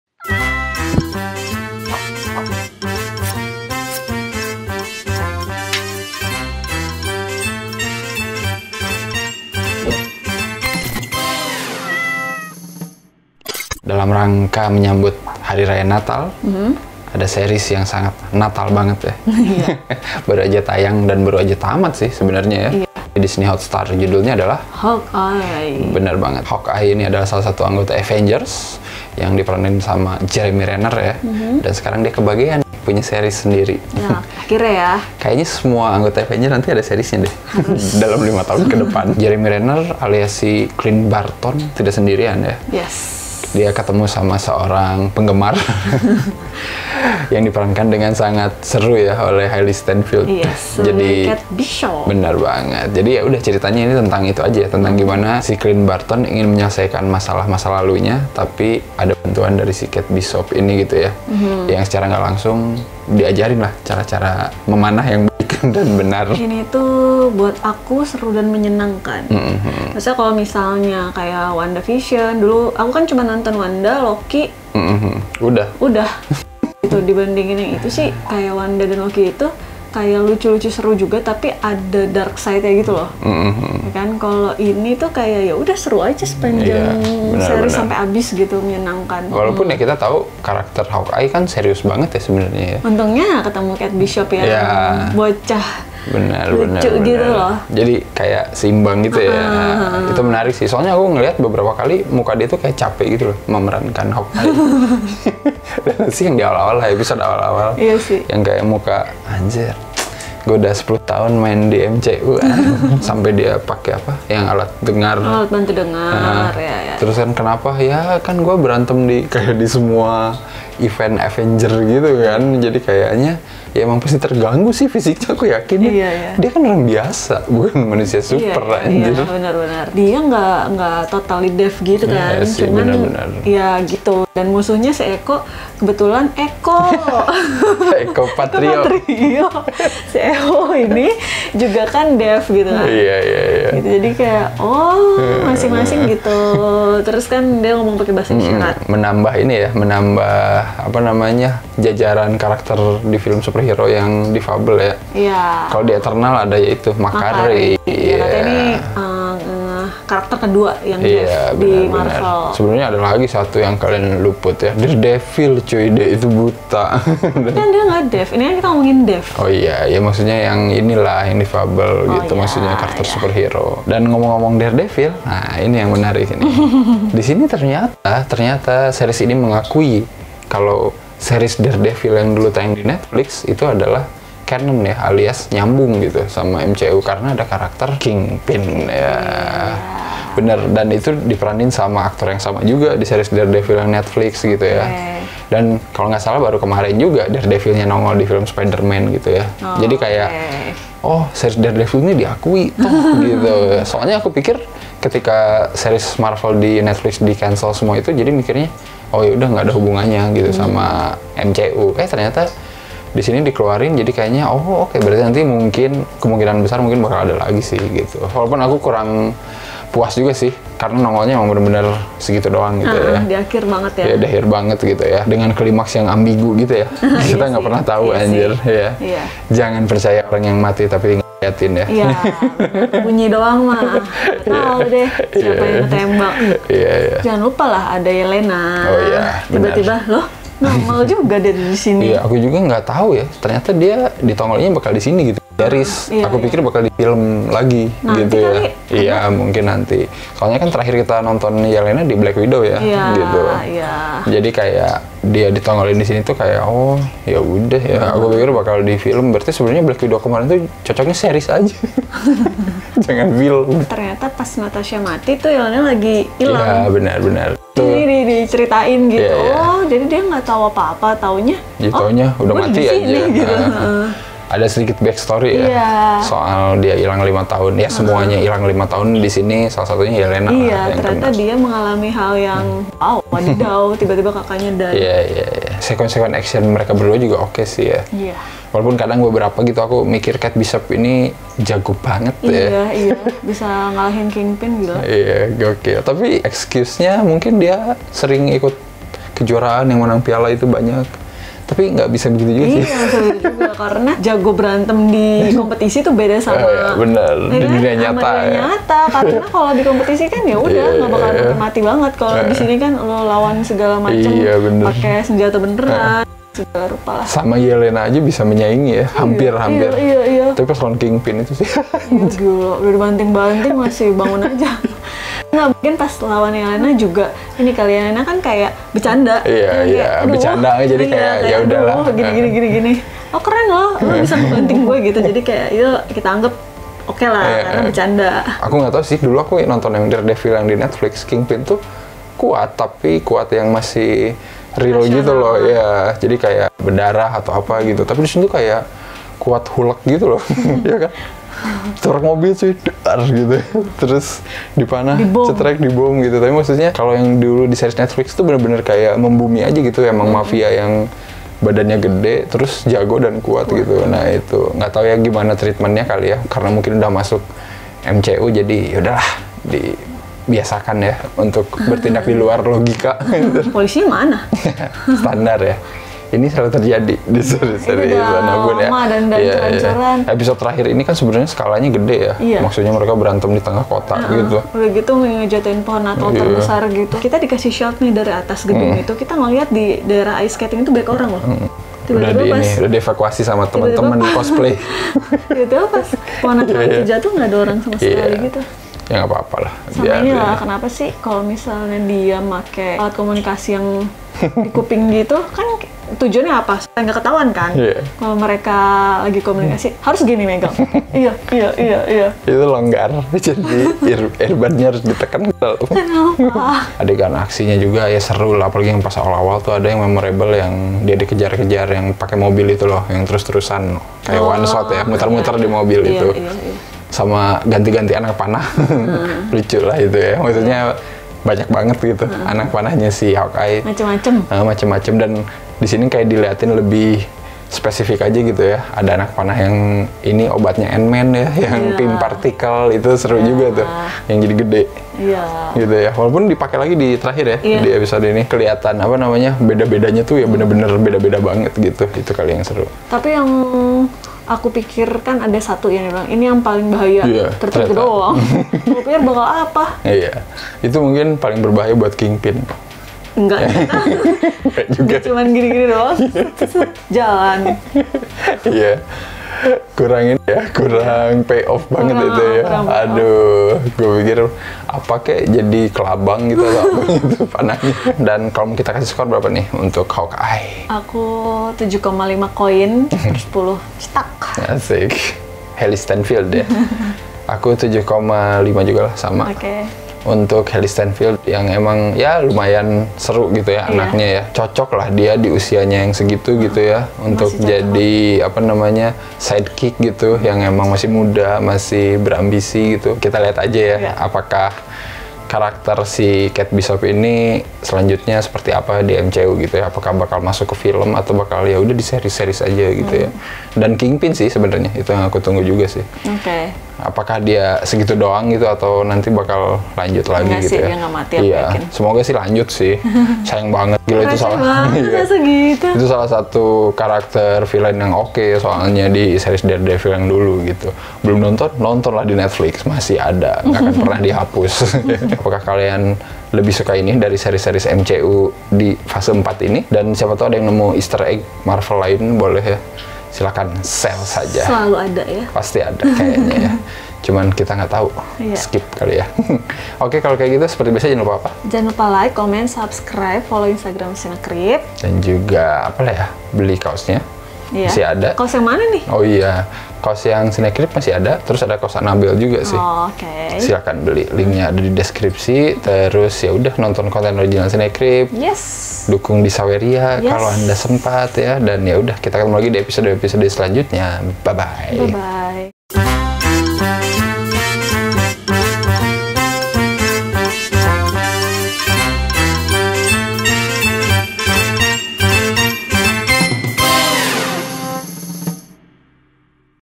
Review HAWKEYE yang Melahirkan Jagoan Baru dan Mendatangkan Musuh dari Masa Lalu
Host: @ariasparrow @anthywf